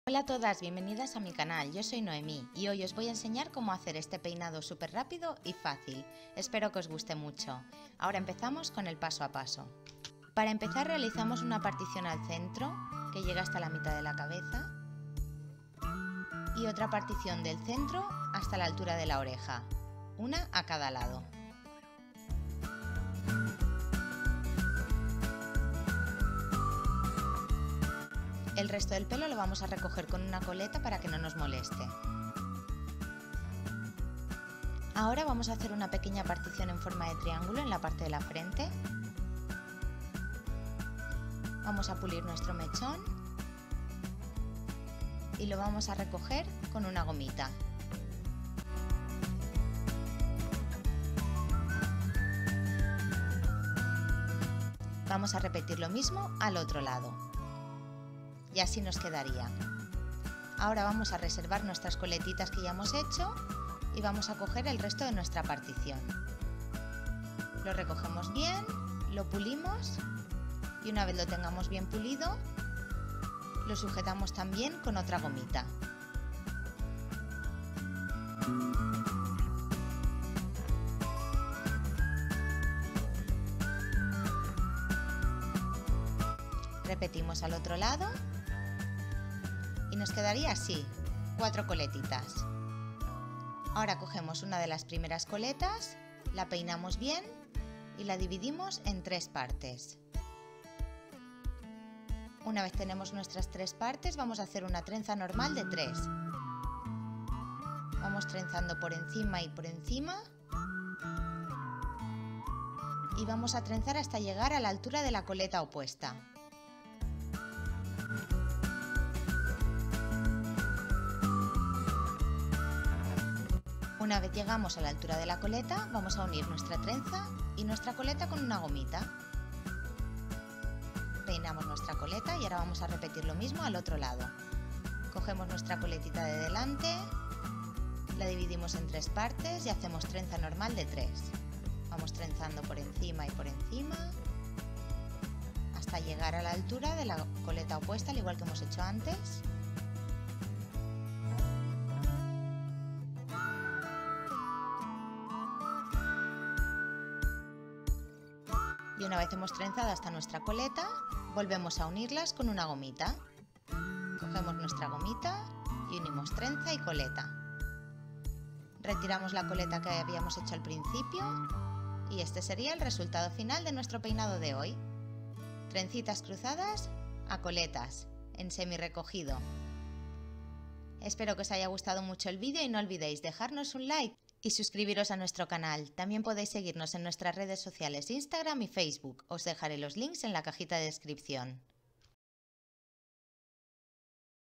PEINADO FACIL Y RAPIDO PARA LA ESCUELA - PEINADOS PARA NIÑAS
HOY OS DEJAMOS ESTE BONITO PEINADO RAPIDO Y FACIL CON TRENZAS ESPERO QUE OS GUSTE MUCHO.

VIDEOS ANTERIORES

VIDEOS ANTERIORES